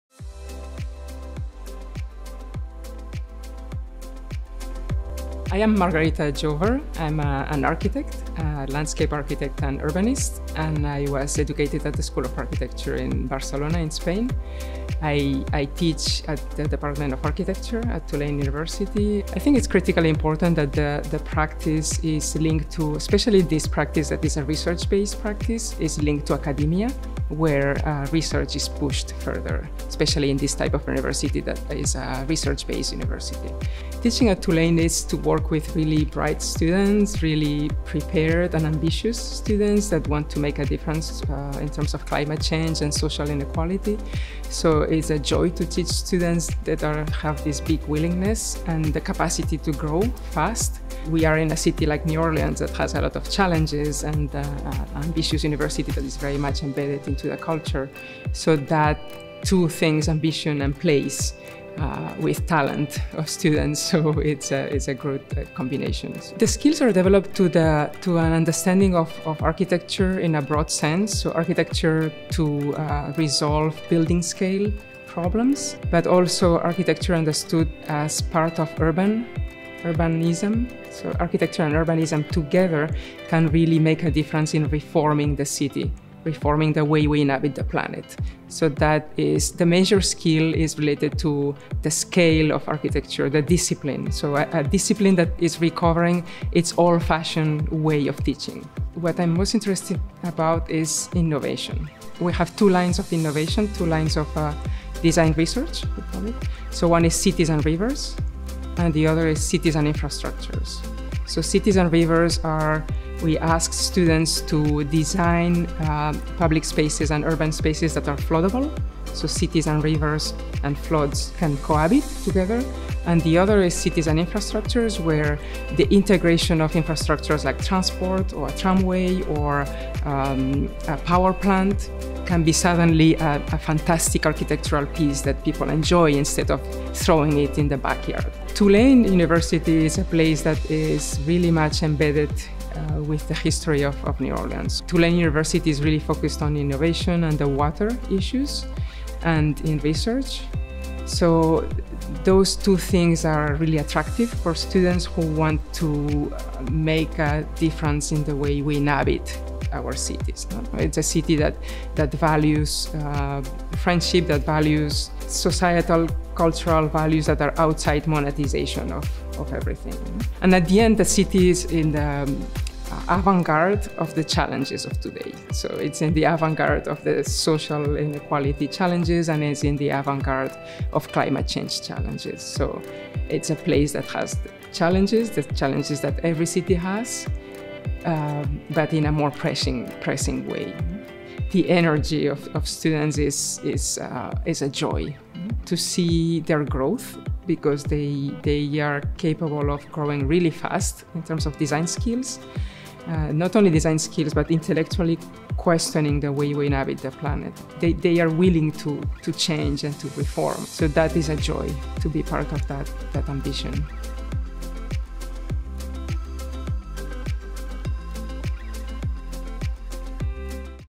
Why Tulane M.Arch - Margarita Jover
Hear from Margarita Jover and discover why you should join the Master of Architecture program at Tulane University!